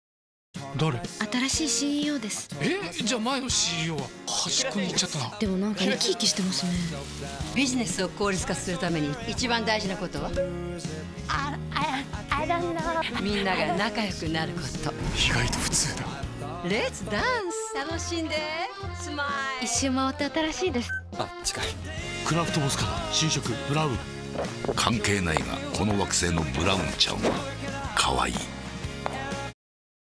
「クラフトボス ブラウン」新CMに夏木マリが登場！「新しい風・新CEO」篇
堺雅人さん、成田凌さん、杉咲花さん、夏木マリさん、小澤慎一朗さん（ピスタチオ）、ゆりやんレトリィバァさん、トミー・リー・ジョーンズが出演する「クラフトボス ブラウン」の新CM「新しい風・新CEO」篇が公開されました。
新CMでは堺雅人さんが働く、とあるIT企業のオフィスのあちこちで、従来の勤務スタイルや価値観からは想像もできない言動＝「新しい風」に遭遇する姿を、個性豊かな豪華キャストとともに表現しています。
今回の最新作では、前回からのお馴染みのキャストに加え、新CEO役として夏木マリさんが加わり、常に携帯する「くまのブラウン」のぬいぐるみと、愛するブラウンカラーで統一した
ゴージャスな衣装とがトレードマークの夏木さんは、社員一同を前にした就任挨拶で、ビジネス効率化のために大事なことは、「みんなが仲良くすること」だと説きます。
いかにも切れ者といった外見からは想像できない、ごく普通の言葉に、「新しい風」を感じる堺さん。
ところが、社内の円滑なコミュニケーションを図るために、社員たちに促したのは、とある意外な行動でした。
従来の価値観からかけ離れたものでありながら、誰もが一度は経験したことのある、ビジネス効率化に最適なプランとは果たして……。
ラストを締めくくる、宇宙人ジョーンズと夏木さんの息の合った決めポーズとともに、見どころ満載の新CMに仕上がっています。